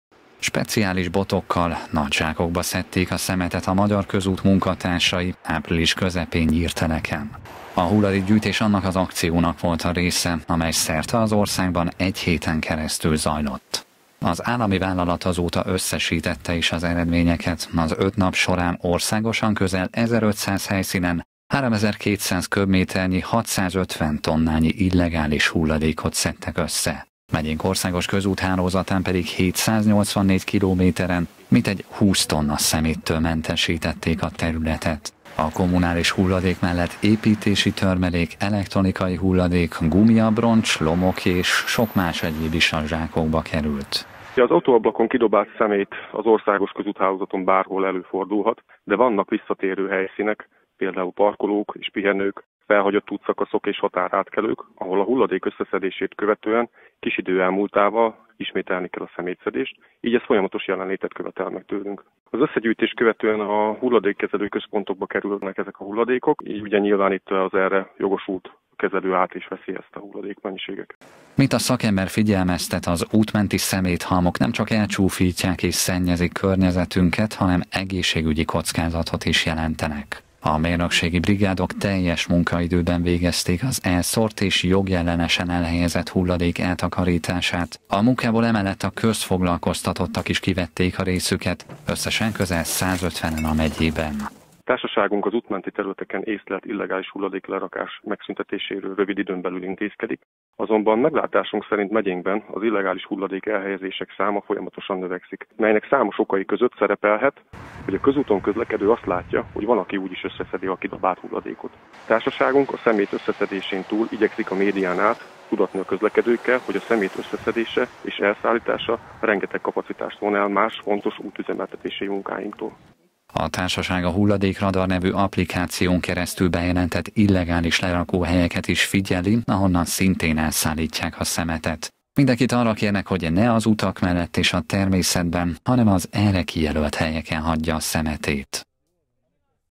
Összeszedték a szemetet
Mintegy húsz tonna szeméttől tisztították meg a megyét a közelmúltban a Közút munkatársai. Az illegálisan lerakott hulladékot hivatalos lerakóhelyre szállítottak el a szakemberek. Megyénkben az utóbbi időben egyre nagyobb méreteket ölt a probléma, sokan ugyanis azt gondolják, hogy bárhol, bármit eldobálhatnak, úgyis lesz, aki összeszedi azt.